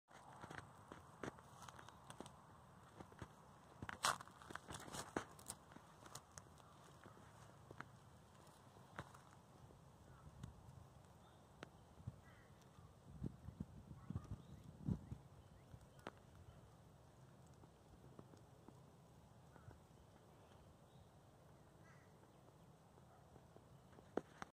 Eastern rat snake after release from plastic garden netting Mar 25 2020
Plastic garden netting is a hazard to wildlife including birds and snakes. Here is a snake that got tangled in the netting. It took over 20 minutes to snip away the netting and free the snake. Eastern rat snake (yellow) aka Pantherophis alleghaniensis.